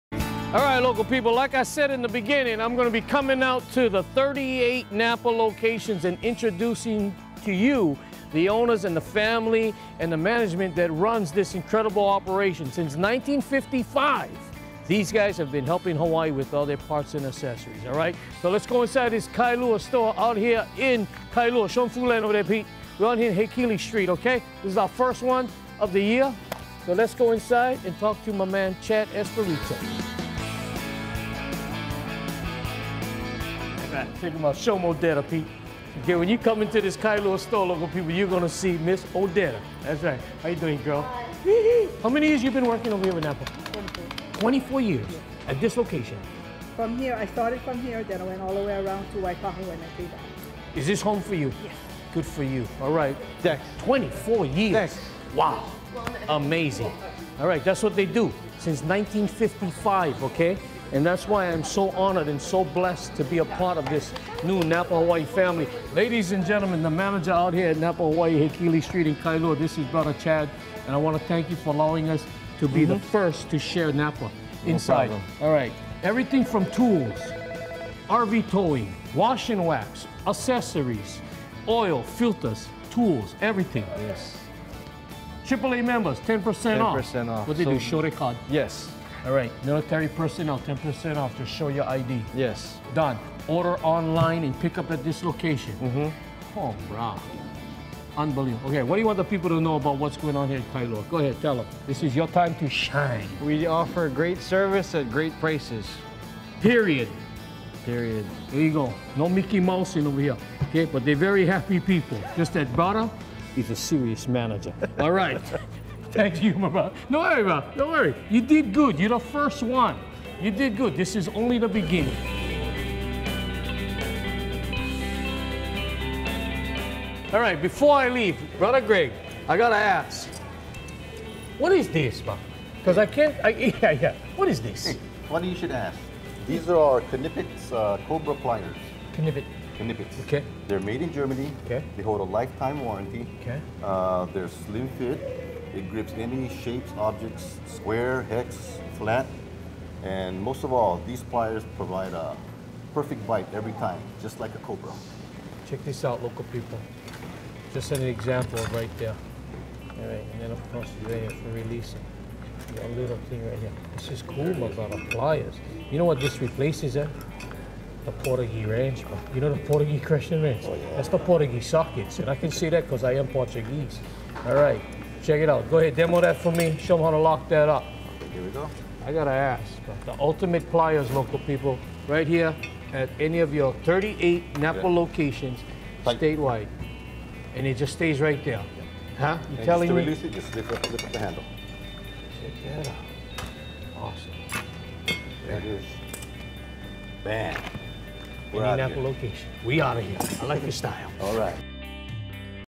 NAPA Hawaii - Kailua
The Champ visits the gang at NAPA Auto Parts Kailua.